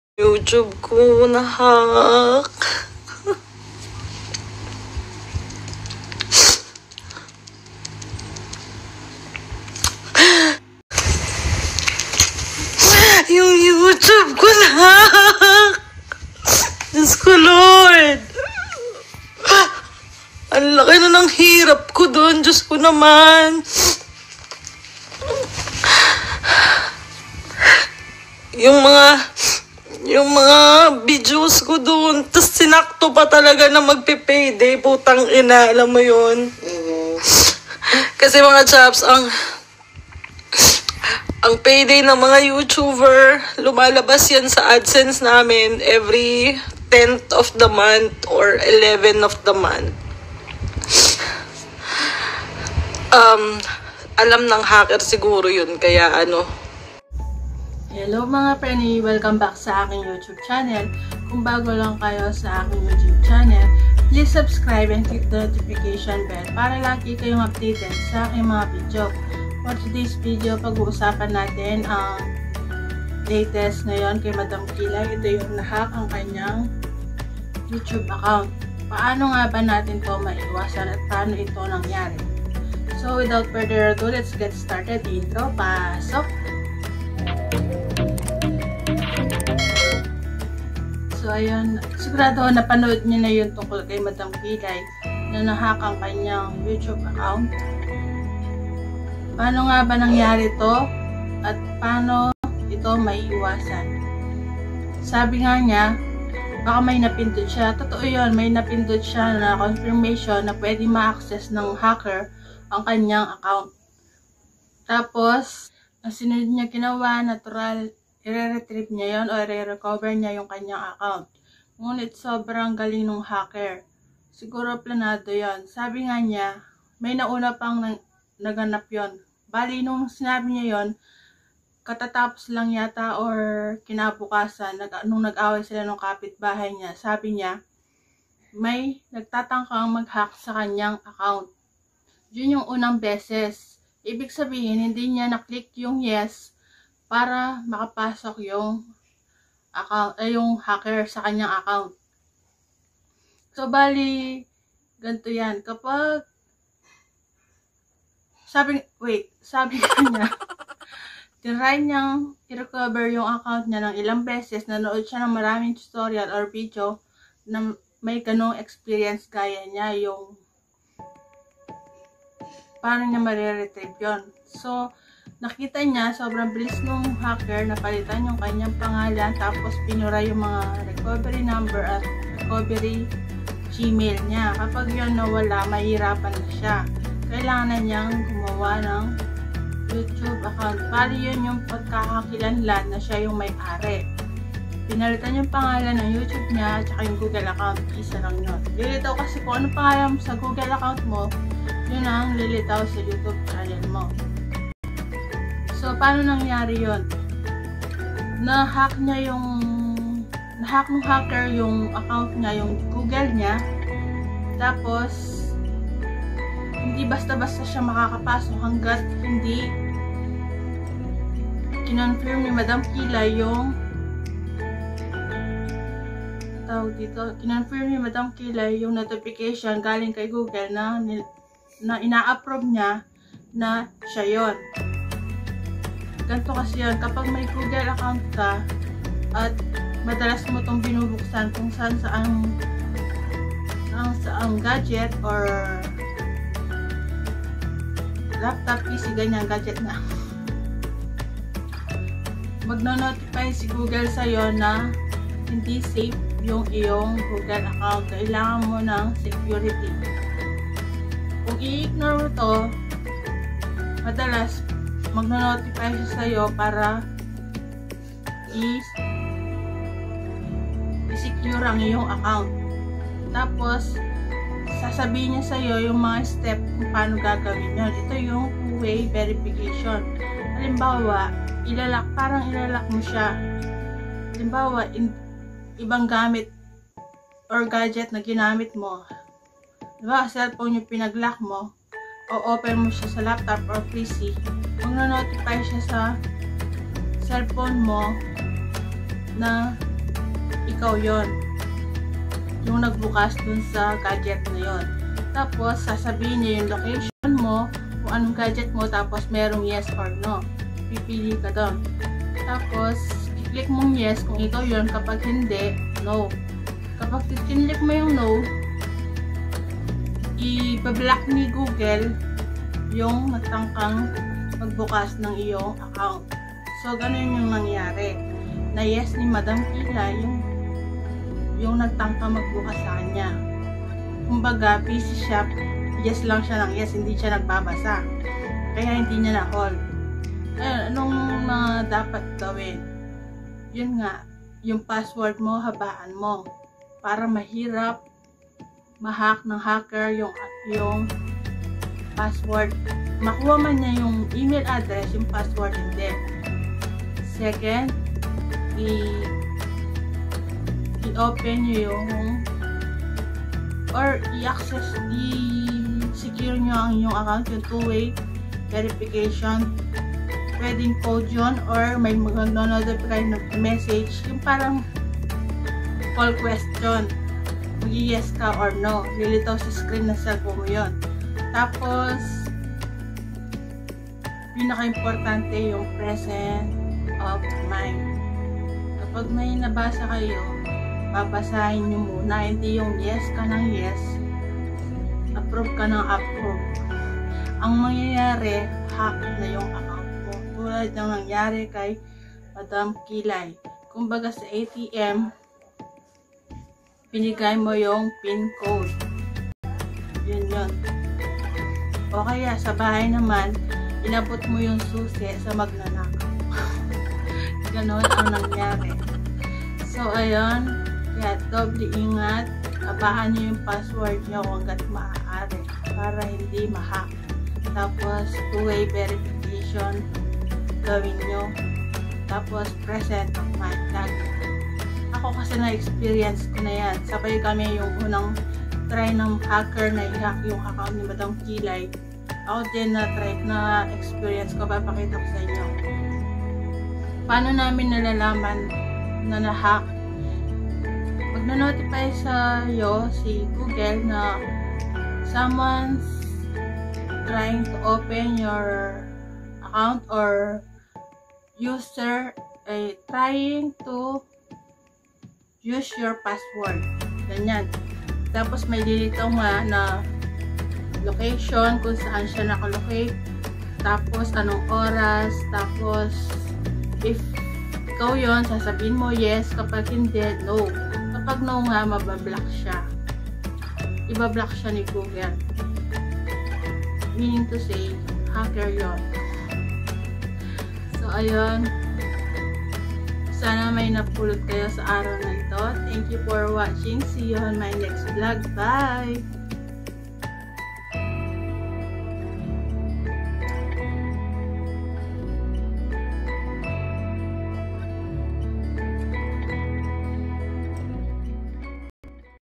YOUTUBE NI MADAM KILAY NAHACKED! PAANO NANGYARE? ANO DAPAT GAWIN?
Madaming tanong ang iba sa pagkahacked ni madam kilay. Sa video nato ipapaliwanag ko ang ibang katanungan at ssabihin ko mga dapat gawin? Nakakalungkot ang nangyari pero nangyari na ito at mahirap na maibalik. Sana may makatulong sa kanya.